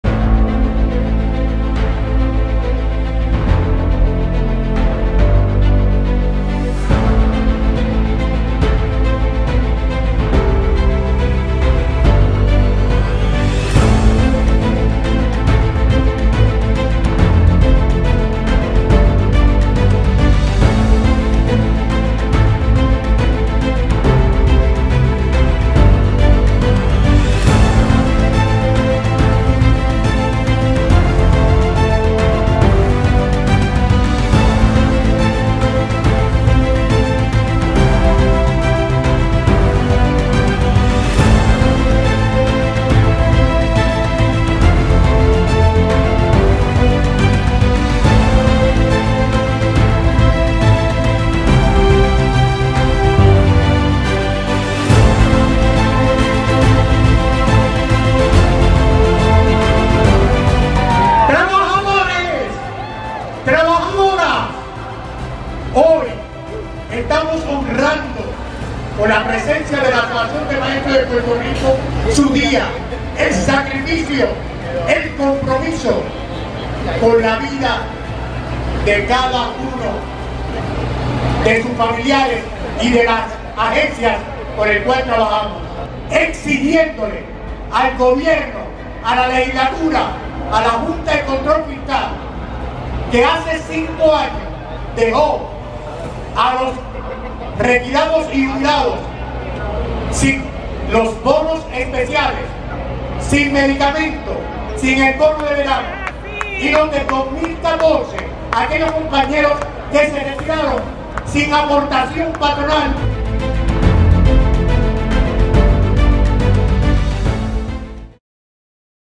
𝐏𝐎𝐑 𝐒𝐈 𝐓𝐄 𝐋𝐎 𝐏𝐄𝐑𝐃𝐈𝐒𝐓𝐄 | 𝑹𝒆𝒄𝒍𝒂𝒎𝒐𝒔 𝑫𝒊́𝒂 𝑰𝒏𝒕𝒆𝒓𝒏𝒂𝒄𝒊𝒐𝒏𝒂𝒍 𝒅𝒆 𝒍𝒐𝒔 𝑻𝒓𝒂𝒃𝒂𝒋𝒂𝒅𝒐𝒓𝒆𝒔
Con el firme propósito de defender a los maestros retirados, que en los últimos años han visto reducciones en sus pensiones, después de la aprobación del Plan de Ajuste de la Deuda y la eliminación de muchos beneficios de retiro, la Asociación de Maestros de Puerto Rico junto a su 𝐃𝐞𝐩𝐚𝐫𝐭𝐚𝐦𝐞𝐧𝐭𝐨 𝐝𝐞 𝐌𝐚𝐞𝐬𝐭𝐫𝐨𝐬 𝐏𝐞𝐧𝐬𝐢𝐨𝐧𝐚𝐝𝐨𝐬 𝐲/𝐨 𝐑𝐞𝐭𝐢𝐫𝐚𝐝𝐨𝐬 se unió a las actividades en conmemoración del Día Internacional del Trabajador.

Nuestros reclamos fueron:
✅ que se devuelva a los maestros retirados el pago del bono de medicamentos, el bono de navidad y la aportación al plan médic.
✅ que los maestros retirados reciban un aumento en sus pensiones.
✅ que se eleve la aportación de exención contributiva de los maestros retirados de $11,000 a $25,000.
✅ que no se aumente más las tarifas de servicios esenciales y la canasta de alimentos.
✅ que se le haga justicia salarial a todos los trabajadores y trabajadoras.

Seguiremos EnPieDeLucha por nuestros maestros retirados y buscando se le haga verdadera justicia a toda nuestra clase trabajadora.